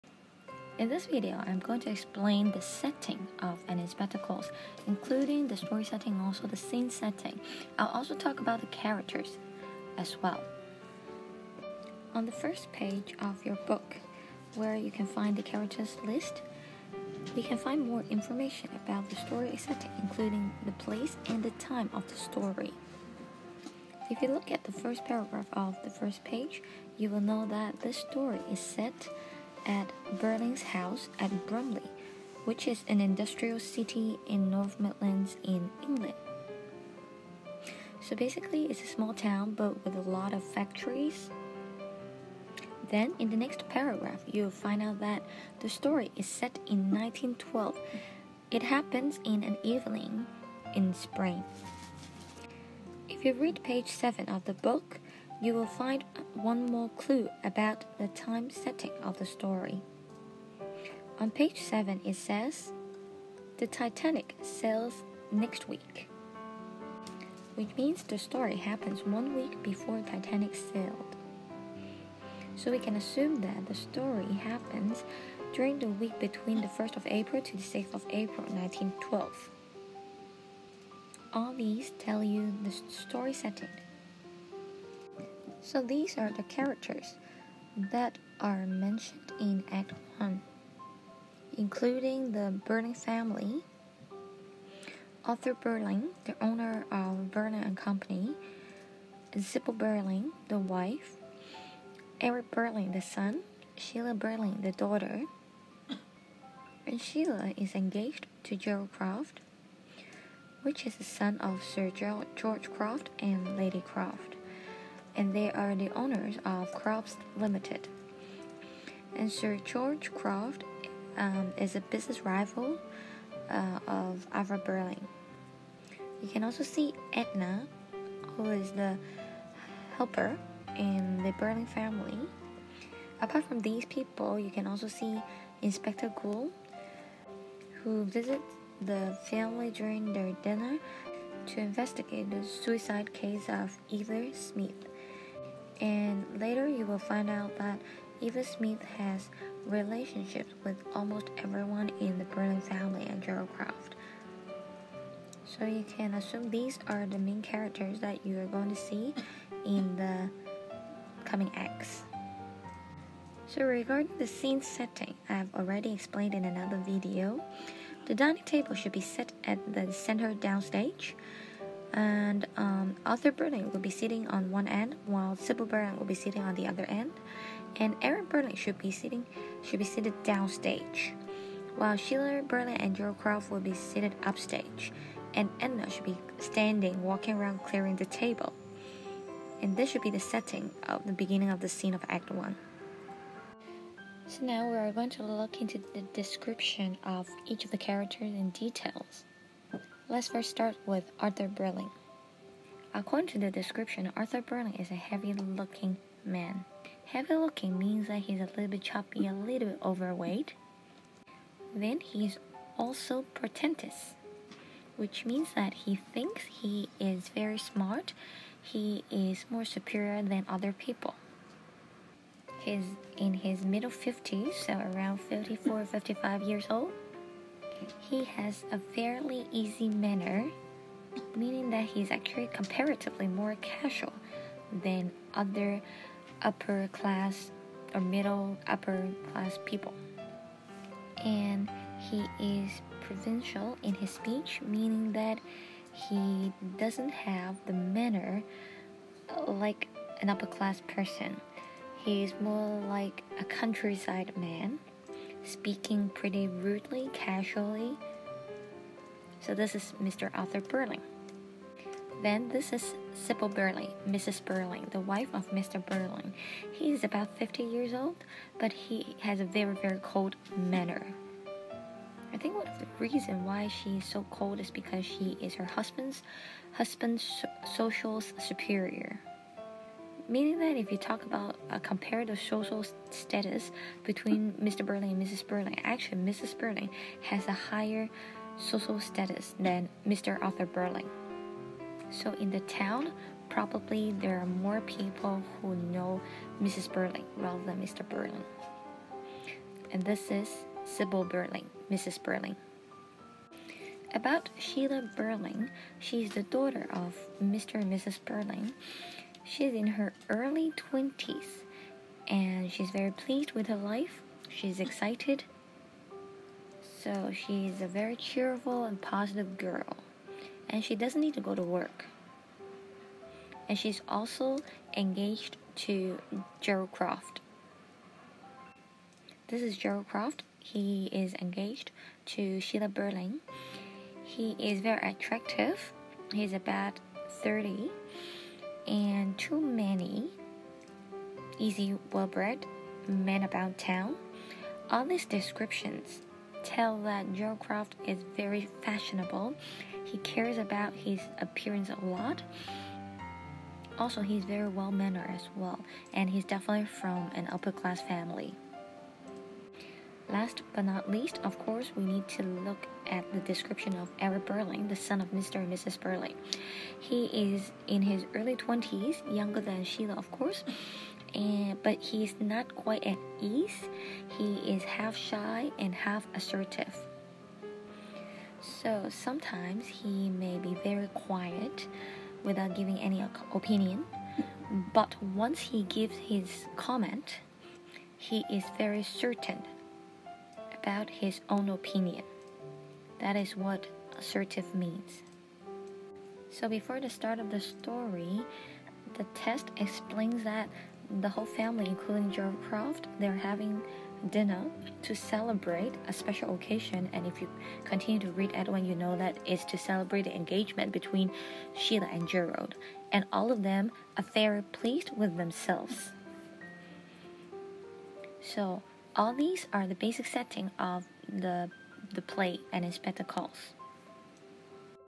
An Inspector Calls - Setting (P.1-2)
Here is the video about the setting of An Inspector Calls for your revision! Hope it helps you understand the background of the story better!